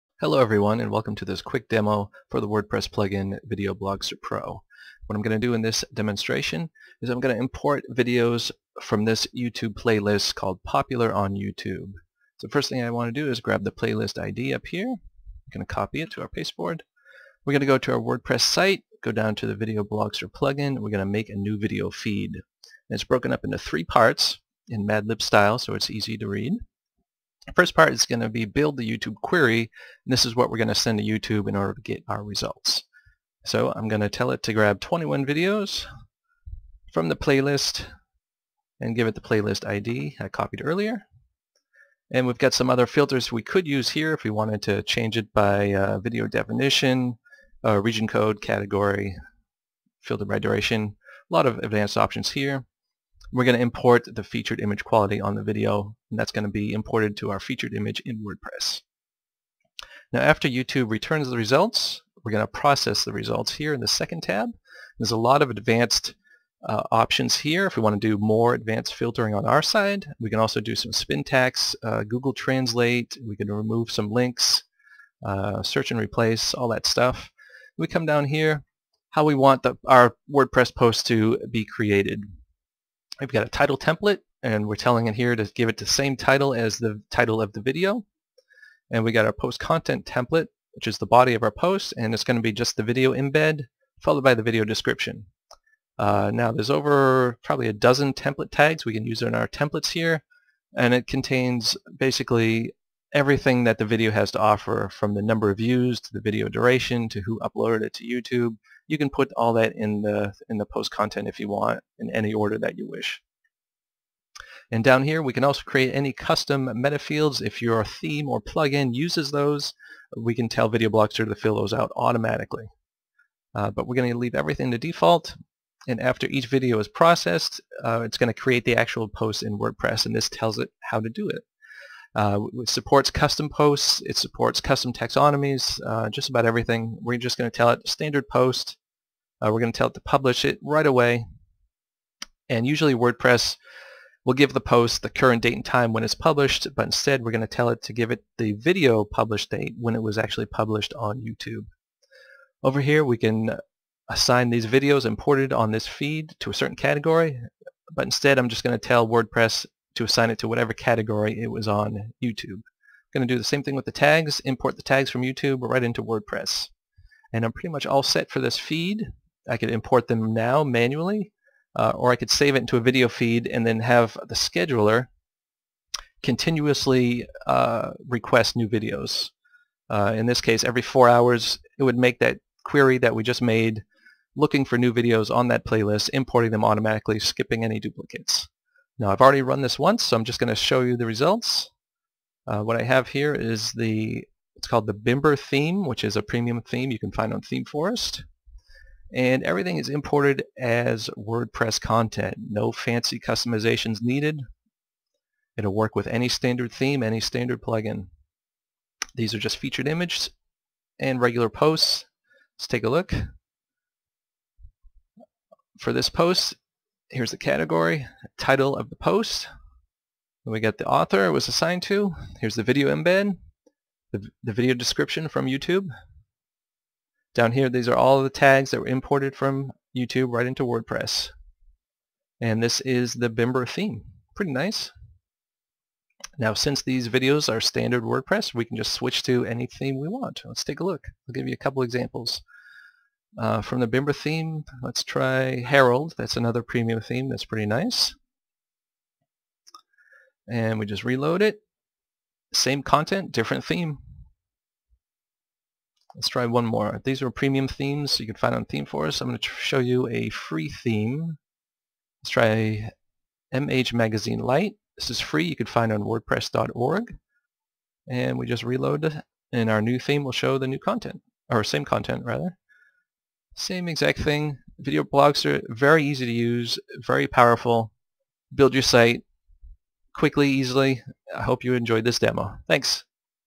Video Blogster 3.0 demo
A demonstration of WordPress plugin Video Blogster Pro, importing videos from YouTube playlist into a website using different themes.